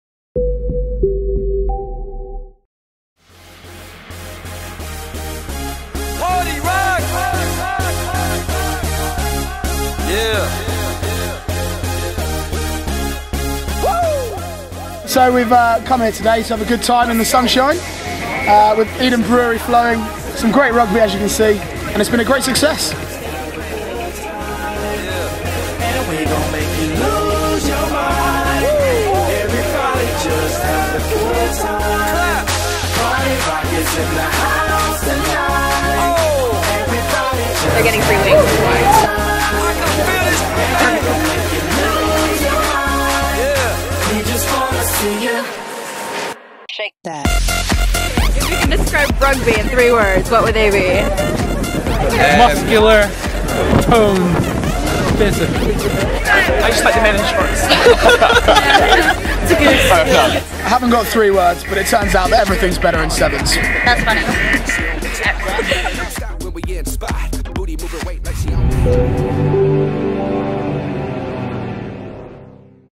Rugby 7s St Andrews 2013
BubbleTV covers Rugby 7s St Andrews 2013!

Produced by Hana Mufti
Filmed by Sam Valles, Winnie Gibb, and Hana Mufti